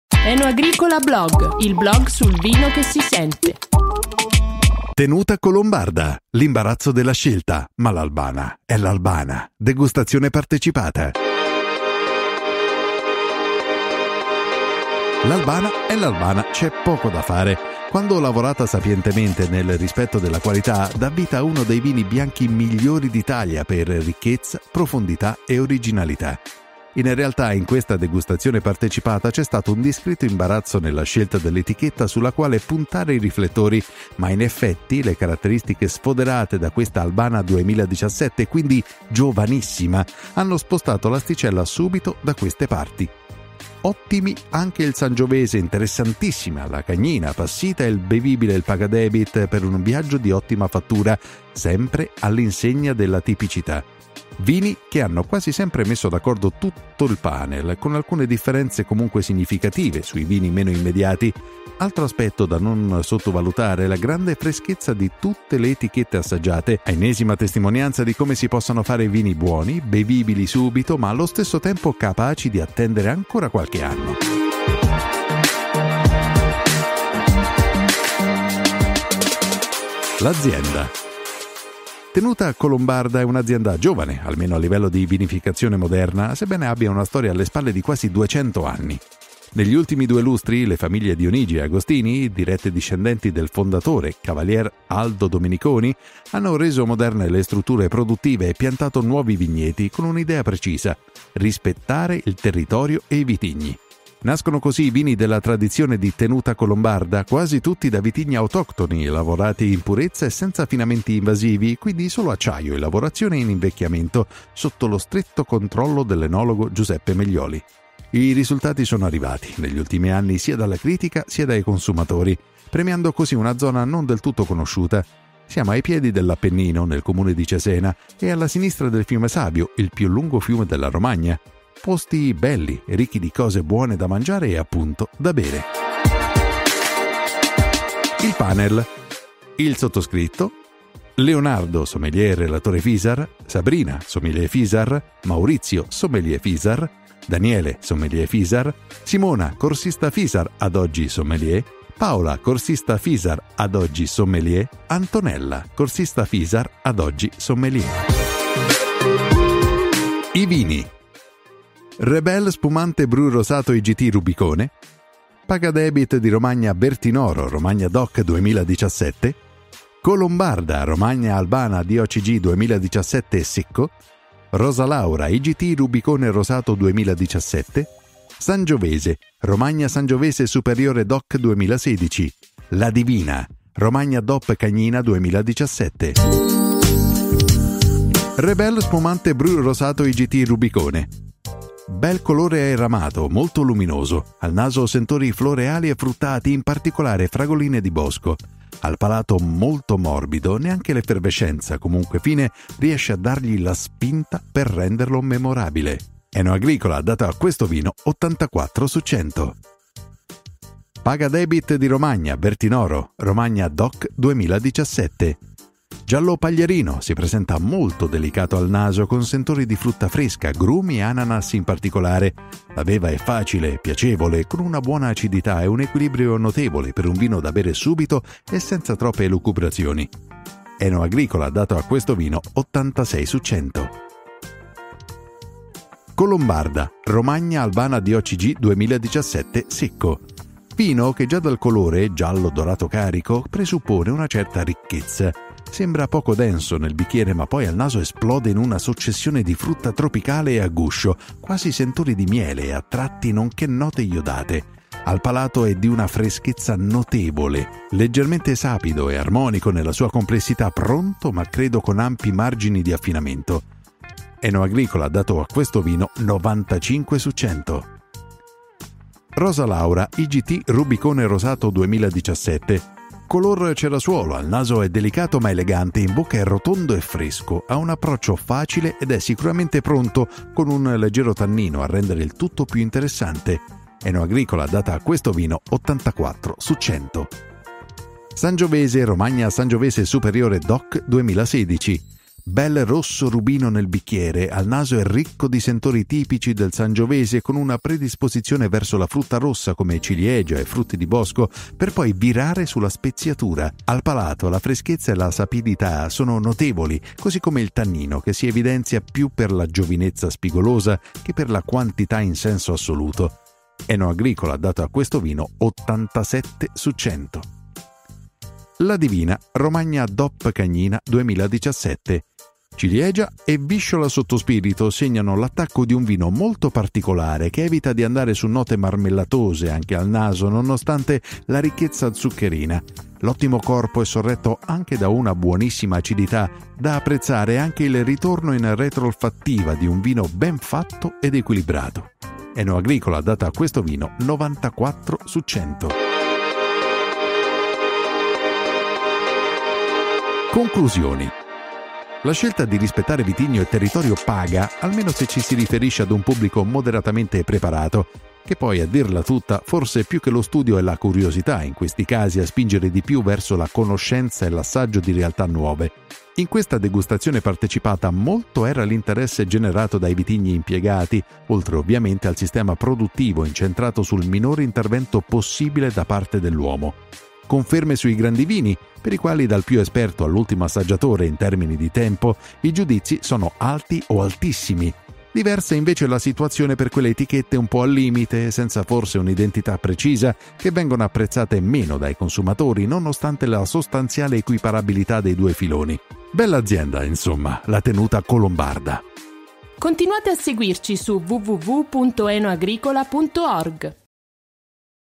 Tenuta Colombarda, l’imbarazzo della scelta (ma l’Albana è l’Albana!) – Degustazione partecipata (cr
L’Albana è l’Albana, c’è poco da fare. Quando lavorata sapientemente e nel rispetto della qualità, dà vita ad uno dei vini bianchi migliori d’Italia per ricchezza, profondità e originalità. In realtà in questa Degustazione Partecipata dedicata ai vini di Tenuta Colombarda c’è stato un discreto imbarazzo nella scelta dell’etichetta sulla quale puntare i riflettori, ma in effetti le caratteristiche sfoderate da questa Albana 2017 (quindi giovanissima) hanno spostato l’asticella subito da queste parti. Ottimi anche il Sangiovese, interessantissima la Cagnina passita e molto bevibile il Pagadebit, per un viaggio di ottima fattura, sempre all’insegna della tipicità. Vini che hanno quasi sempre messo d’accordo tutto il panel, con alcune differenze comunque significative sui vini meno immediati. Altro aspetto da non sottovalutare la grande freschezza di tutte le etichette assaggiate, a ennesima testimonianza di come si possano fare vini buoni, bevibili subito ma allo stesso tempo capaci di attendere ancora qualche anno.